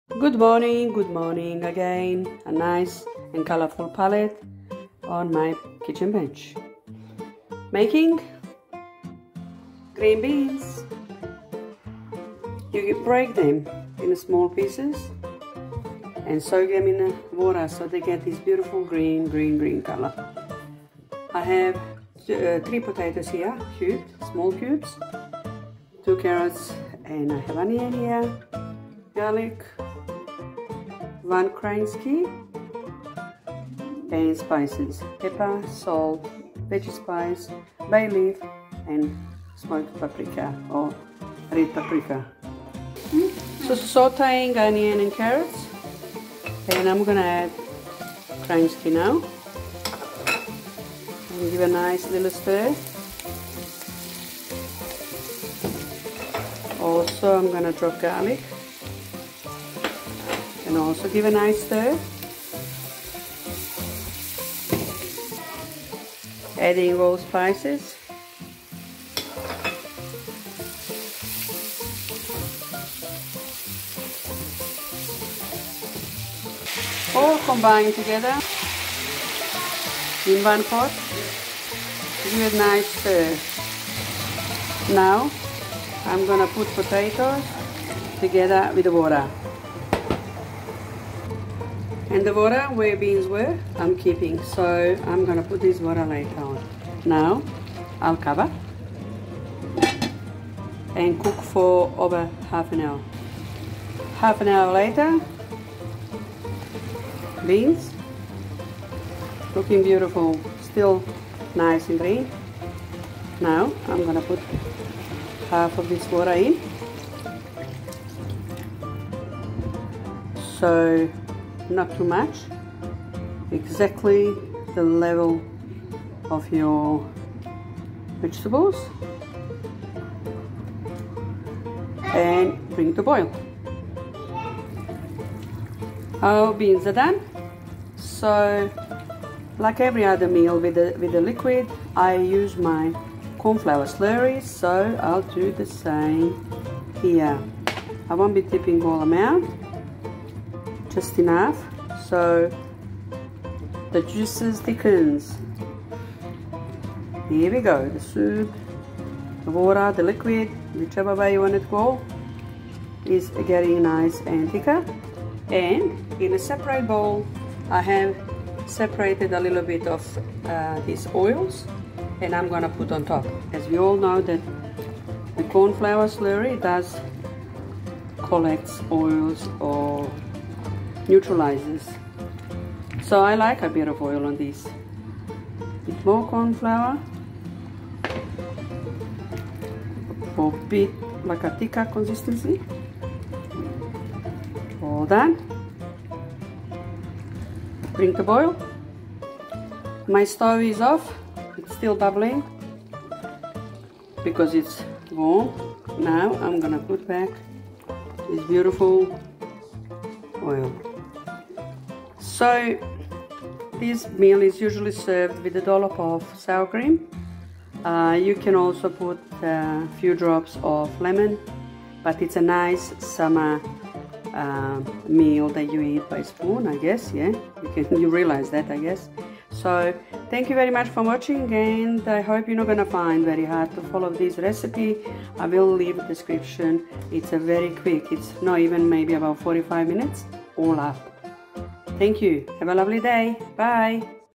Green beens /Summer dish
Beans...what to do with them..usually  getting  them stemmed  or boild ...So how about dish that is so light and  easy..
Ingredients:
1 kg beens (green or butter beens  even better)
3 medium  potatoes 🥔
2 carrots 🥕
2 small onions
3 garlic cloves
4 tbs oil ( people say not to much fat..wrong  ..Worry about  the  carbs and sugar)
1 Kransky

Spices:
1 /2 tbs salt
1 tbs chicken stock powder  or vegetable stock
1 tsp cracked  pepper
1 tbs red sweet paprika  (I  have not added any chilli for particular  health reason  but you can)
2 bay 🍃
2 tbs corn flour
1 lt  water

Sour cream
Lemon juice